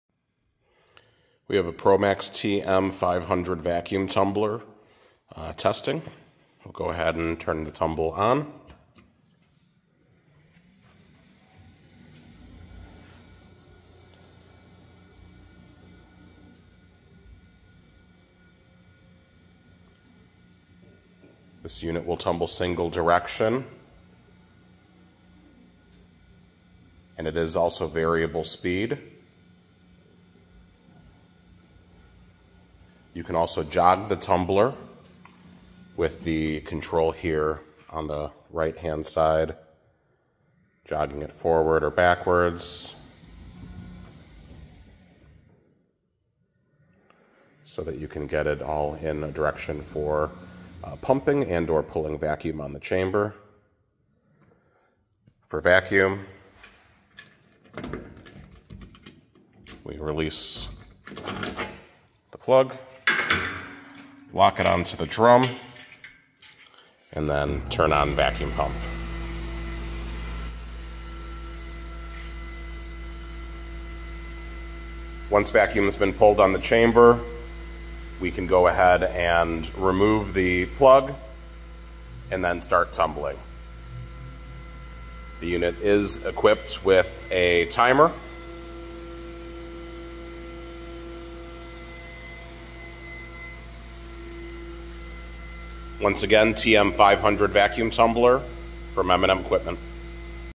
PROMAX TMC-500 Vacuum Tumbler item 9763
Promax 500 Lbs. Vacuum Tumbler, all stainless steel construction, Continuous vacuum during tumbling cycle. Vari-speed drive 1-9 RPM, 8 hour digital timer, Forward and reverse drum function, self contained Busch RB 0021 vacuum pump with liquid trap, 15" discharge height, 12" x 23" opening. 1/2hp drive motor, 110V/1PH/60HZ, 20 Amps. Machine size (L x W x H) 59" x 36" x 65".